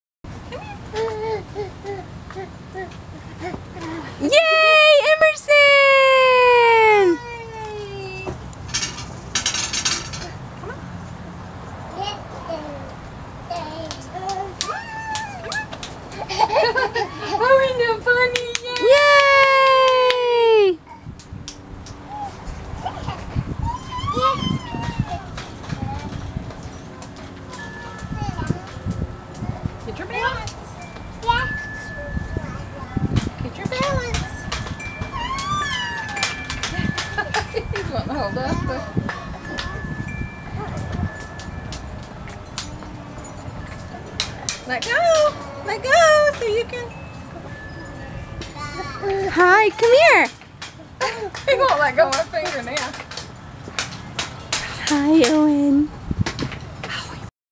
Emerson takes steps
Today Emerson wanted to walk! He is still crawling but today he decided he wanted to take a couple steps while we were out on the playground.